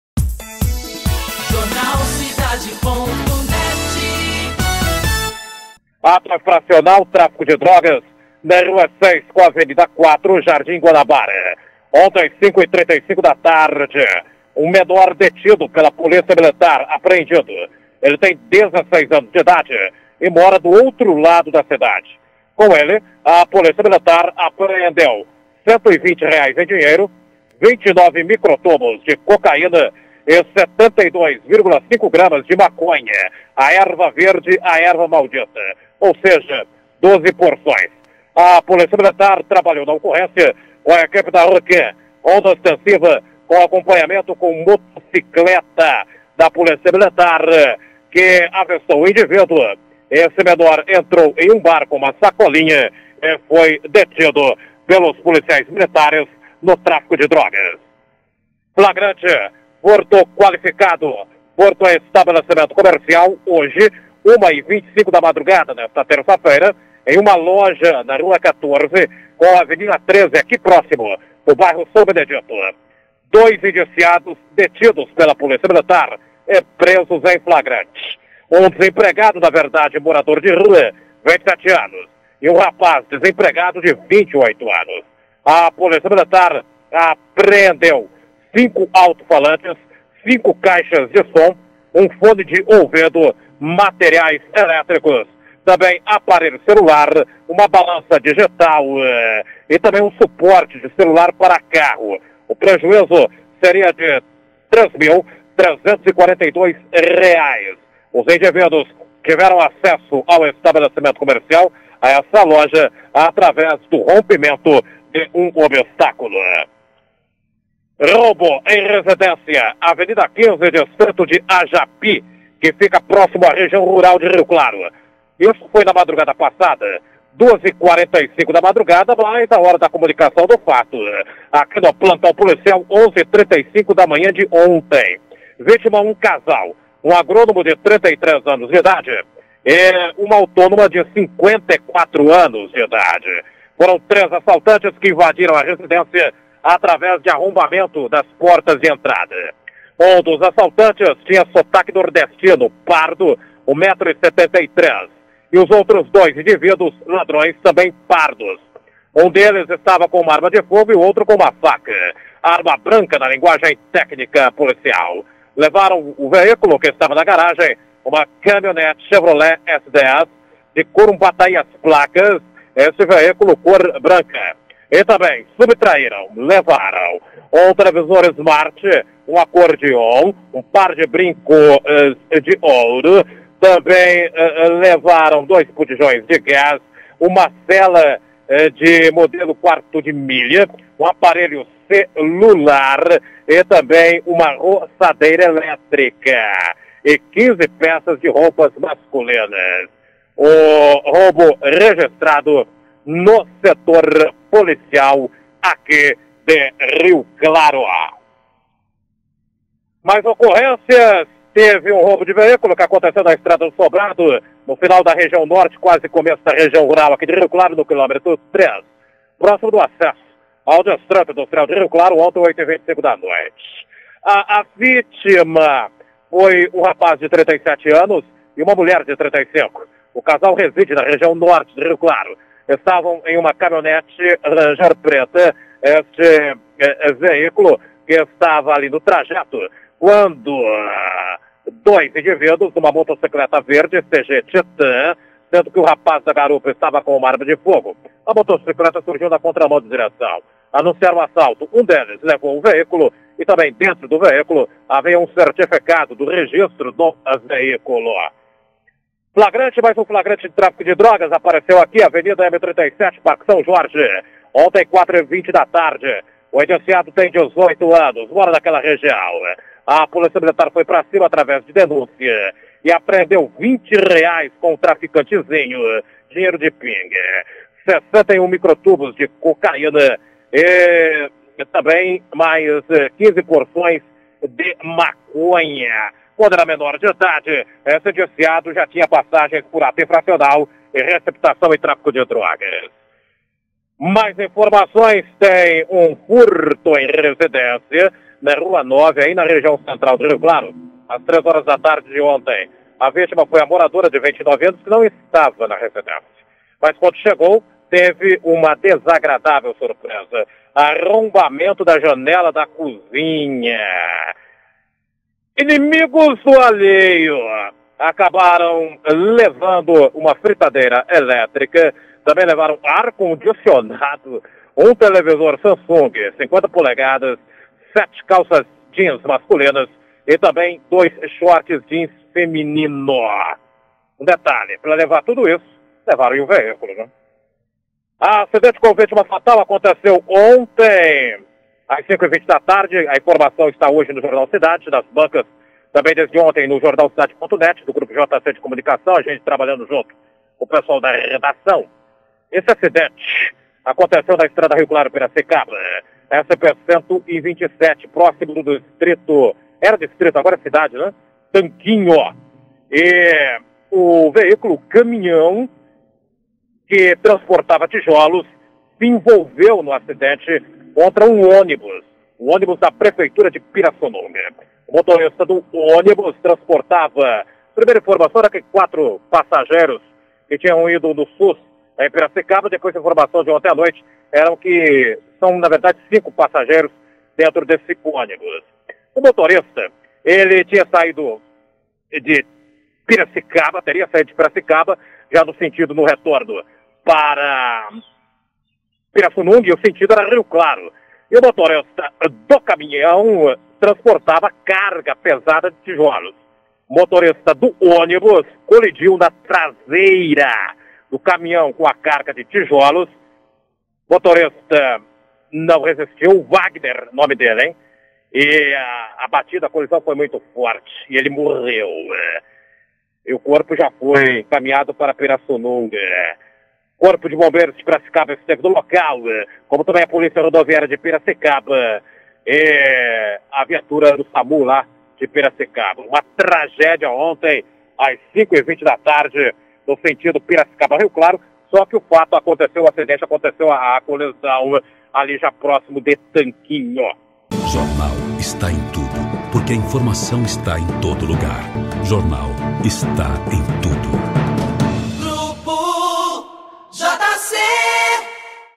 Boletim Policial desta terça-feira (12)
Confira as informações direto do Plantão Policial com o repórter Gilson Santullo.